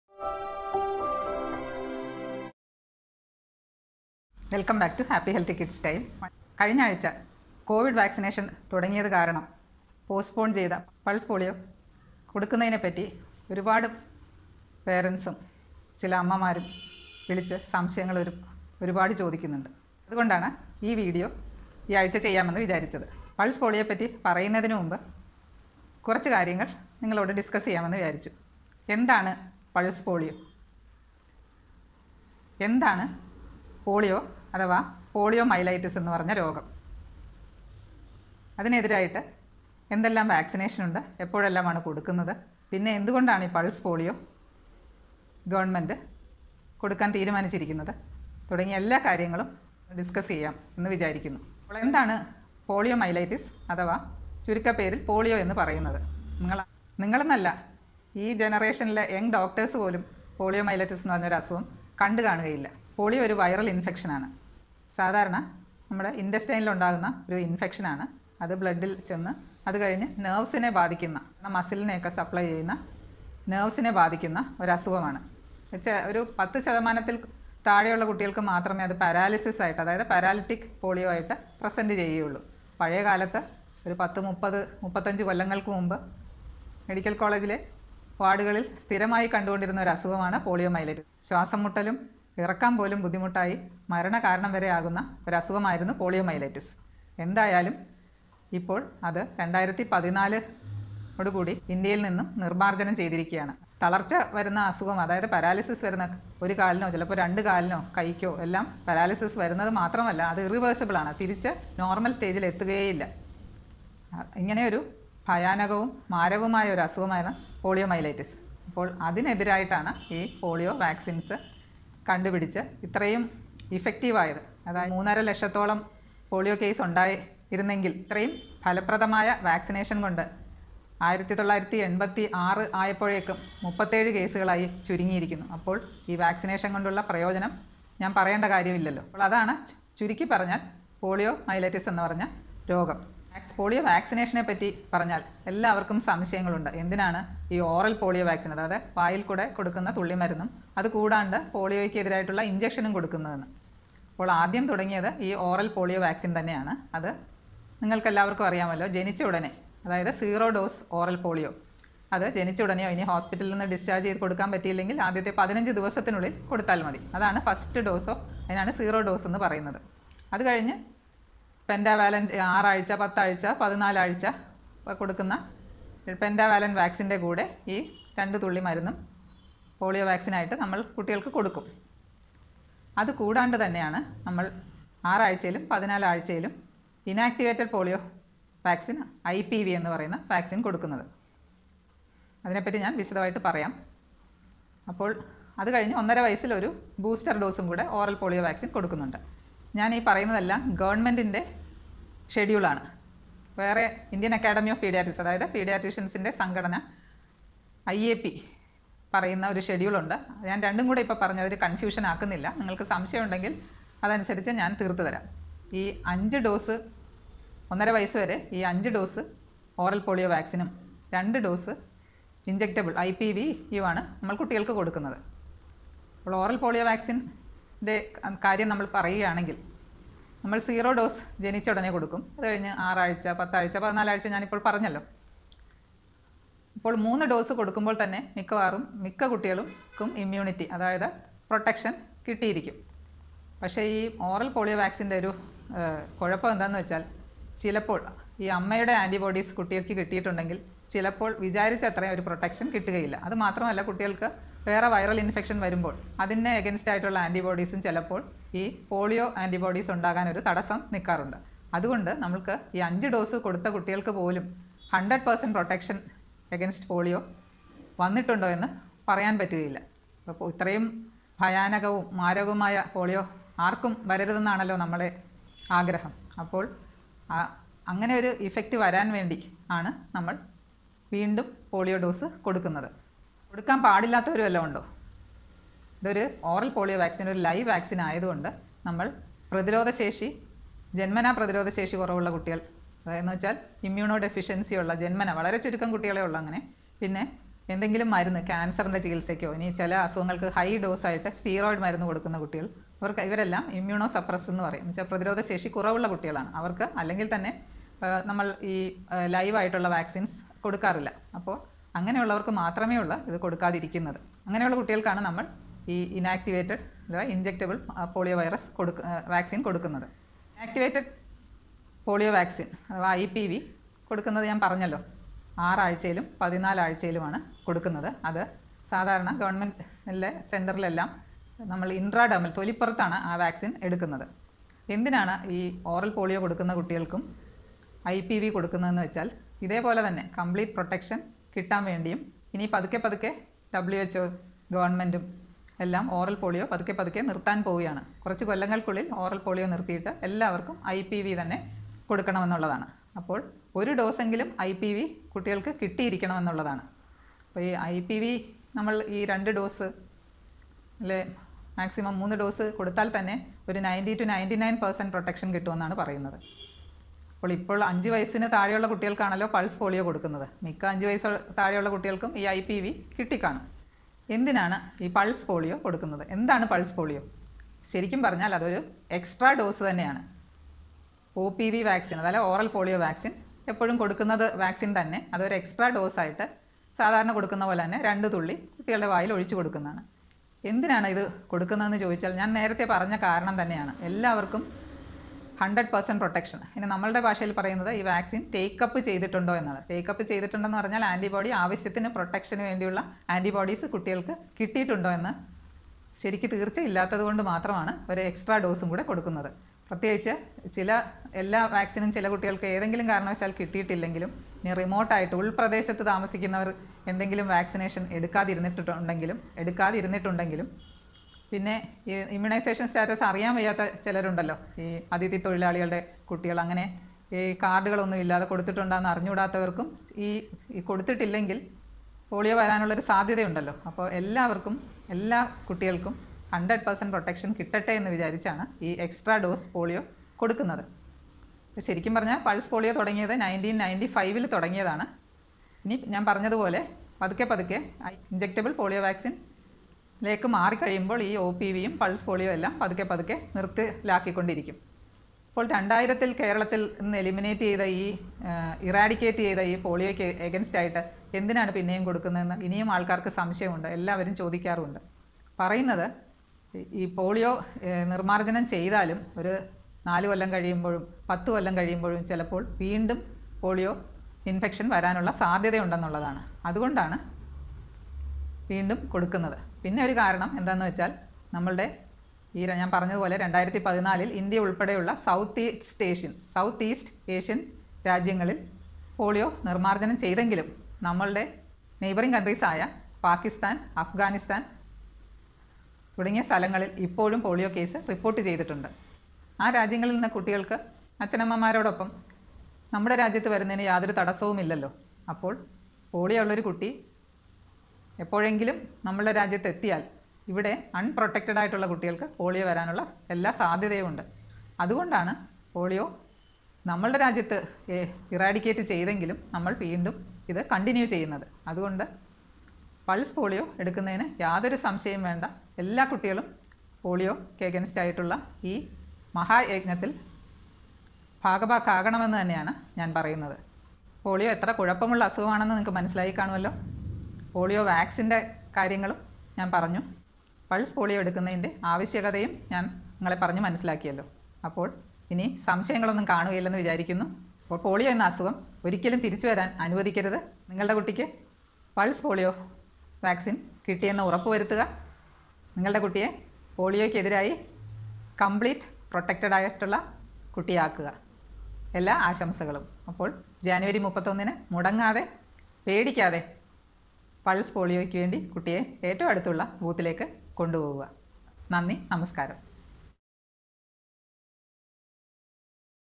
What is OPV, IPV ? Why Pulse Polio Immunisation ?
Queries solved
What is polio or poliomyelitis?
Oral polio vaccine
Inactivated polio vaccine
Why pulse polio vaccine
Encouraging all parents of kids below 5 years to give pulse polio vaccine